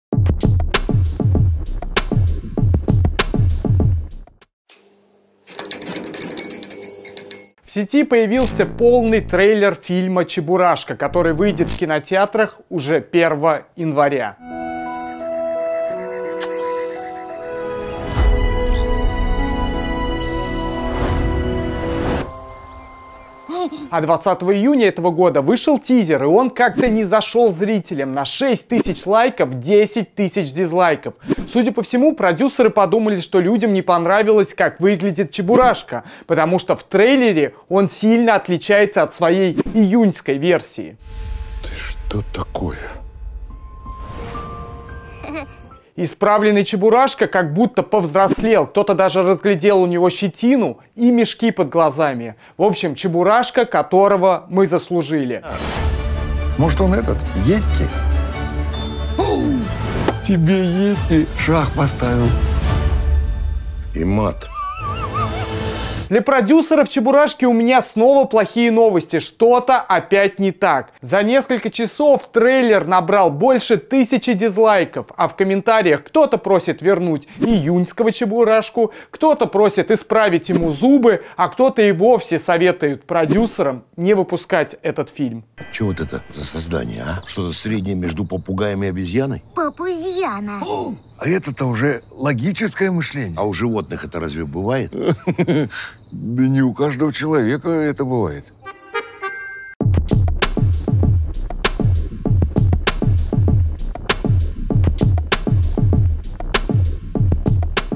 Чебурашку закидали дизлайками | Его перерисовали, но зрителям опять не понравилось
В сети появился полный трейлер фильма «Чебурашка», который выйдет в кинотеатрах уже 1 января. Судя по комментариям и дизлайкам, зрителям снова не понравилось, как выглядит главный герой.